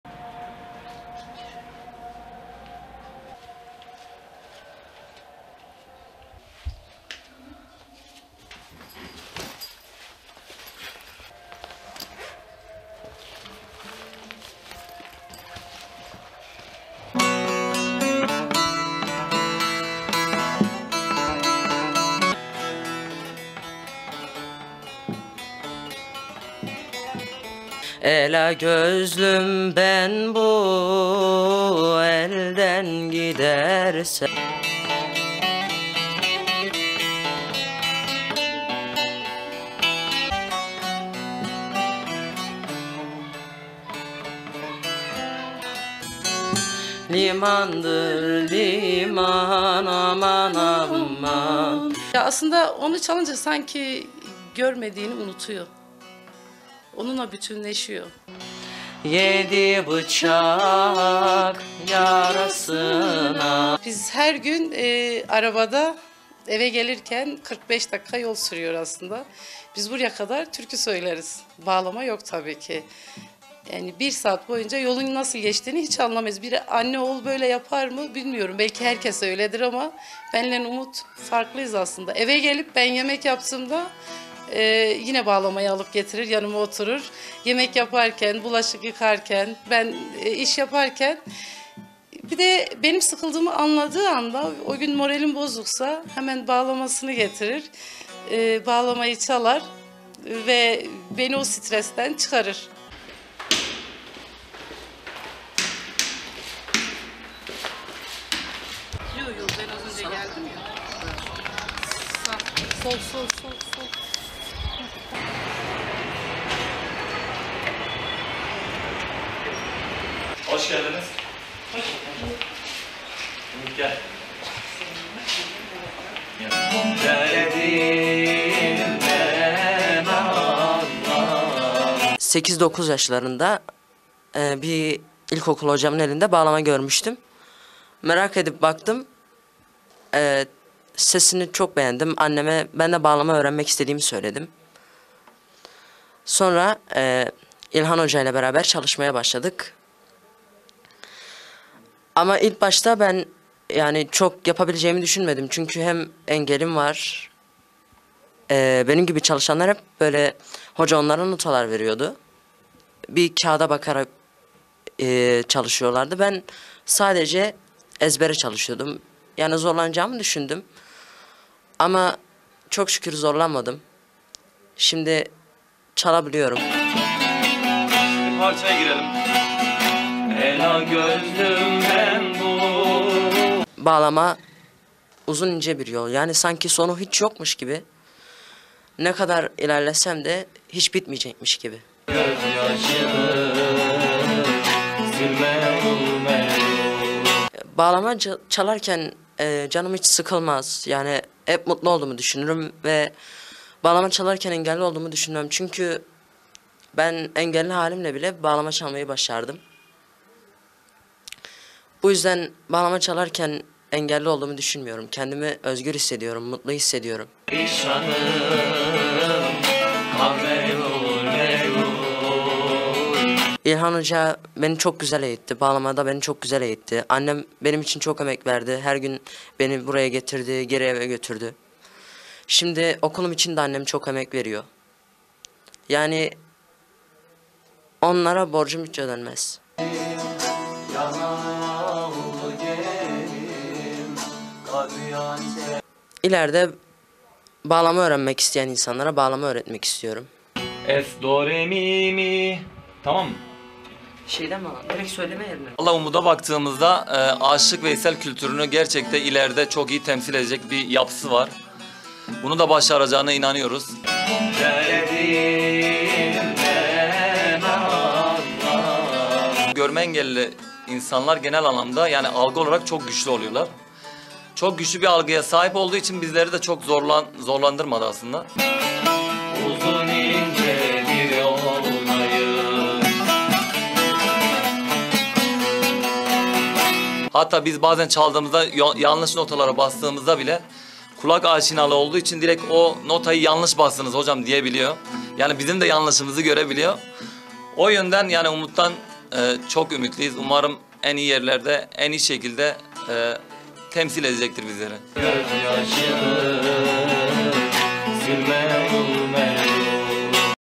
Bağlama Çalan Görme Engelli Umut Eğitmen Olmayı Hedefliyor
Gaziantep'te öğretmeninin derste çaldığı bağlamadan etkilenerek müziğe başlayan 15 yaşındaki Umut Geçili, Güzel Sanatlar Lisesinde eğitim görüyor